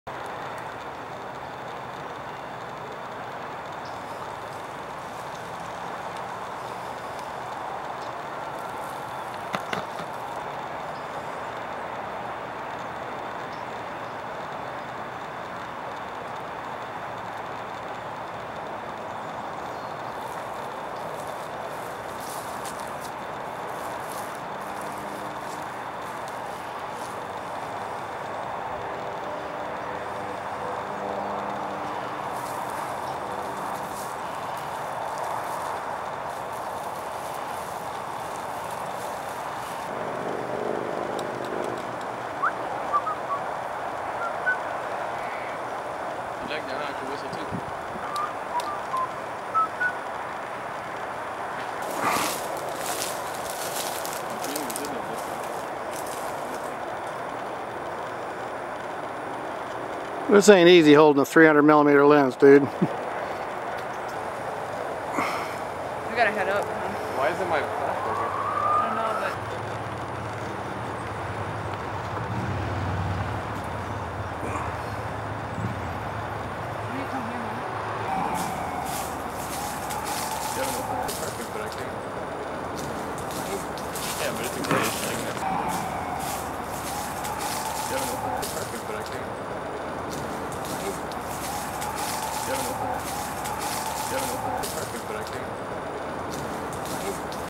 Ash-Throated Flycatcher - Miami-Dade County
Stephen Paez found an Ash-throated Flycatcher near Kendall Indian Hammocks park on February 19, 2011. He posted the information quickly allowing others to get nice looks at this very unusual visitor to Miami-Dade county. Typically several will be seen in the state, but usually they are found north and as far south as Orlando area.